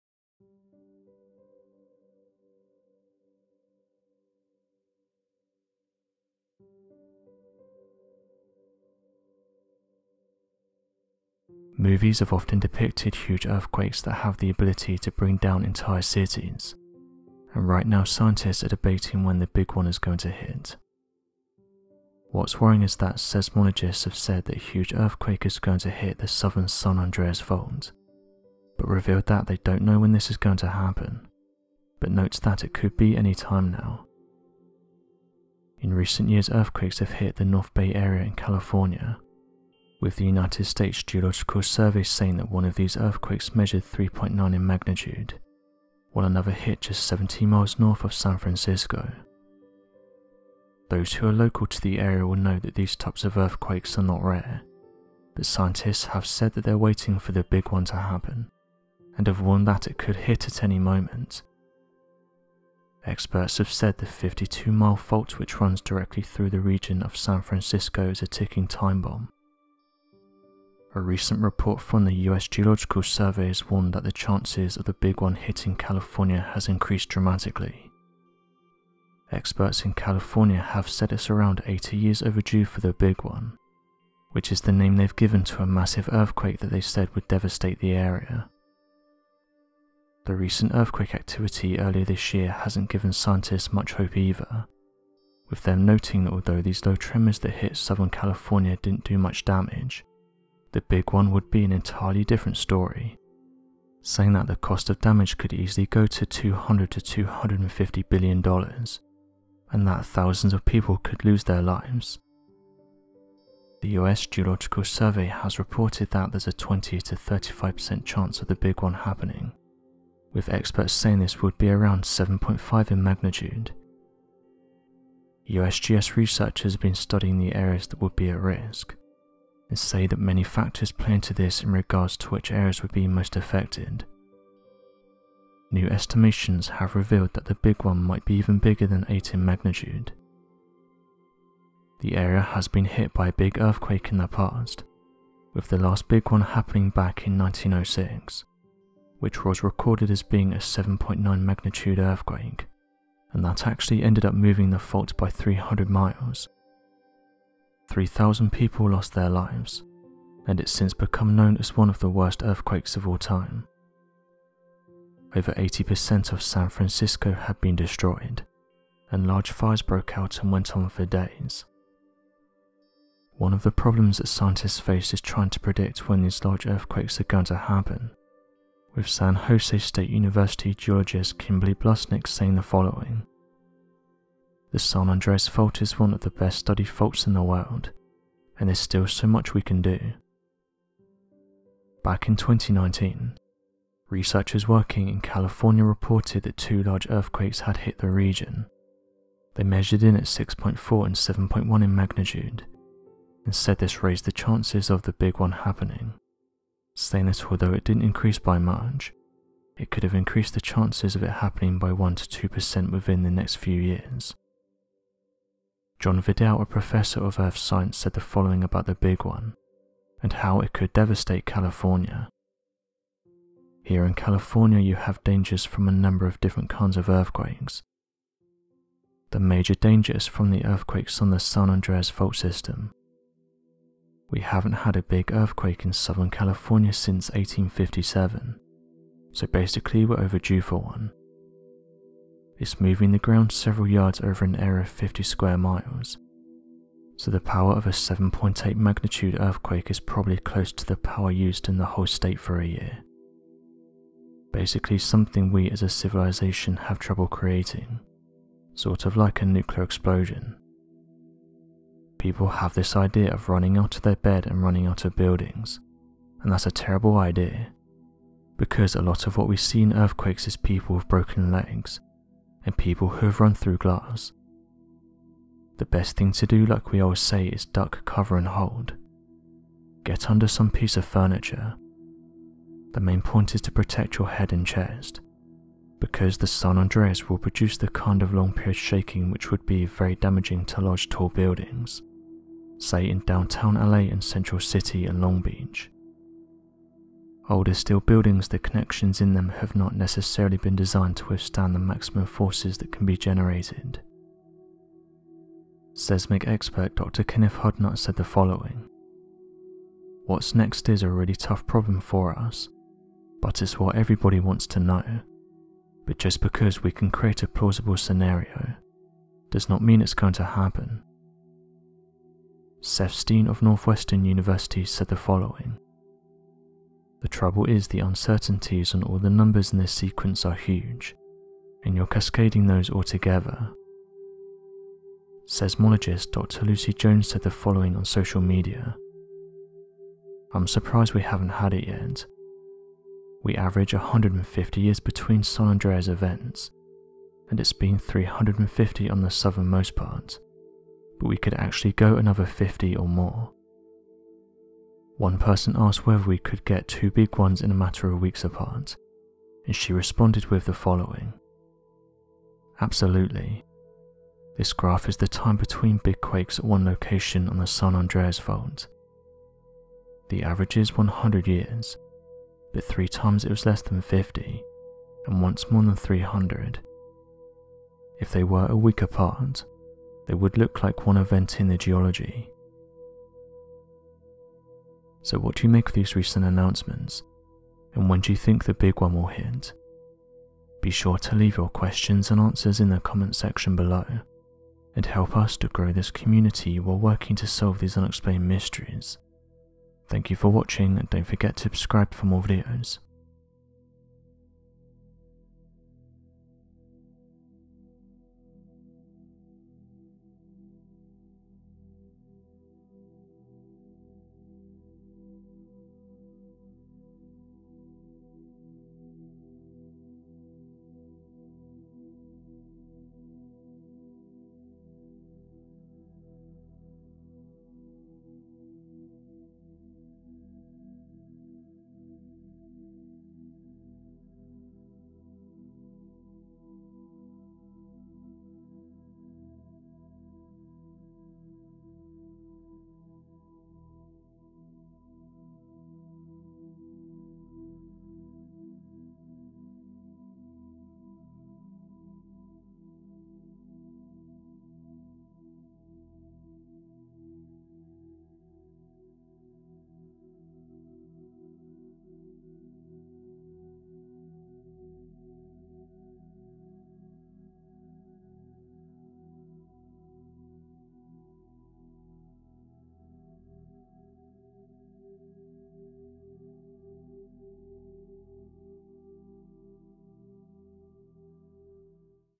Scientists Just Announced The Threat Level Just Increased To High By The U.S. Geological Survey
Scientists just announced the threat level just increased to high by the U.S. geological survey. Today, we take a look at why the US geological survey just increased the threat level to high.

Movies have often depicted huge earthquakes that have the ability to bring down entire cities, and right now scientists are debating when the big one is going to hit. What's worrying is that seismologists have said that a huge earthquake is going to hit the southern San Andreas fault, but revealed that they don't know when this is going to happen, but note that it could be any time now.

In recent years earthquakes have hit the North Bay Area in California.